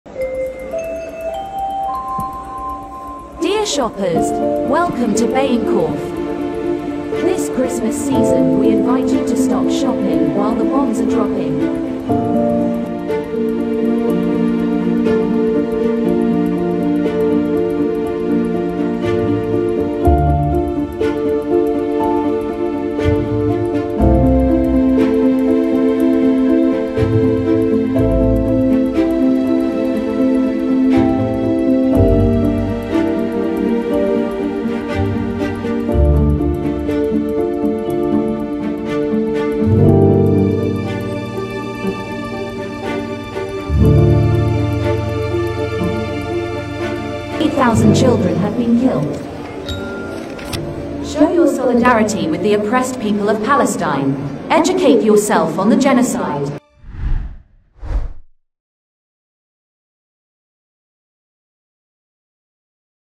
مركز تجاري في هولندا يدعو إلى التضامن مع غزّة
حظي مركز تجاري في أمستردام بتفاعل واسع على مواقع التواصل الاجتماعي، بعد تضامنه مع غزة بطريقة غير متوقعة ودعوته المتسوّقين إلى مقاطعة العلامات الداعمة لإسرائيل.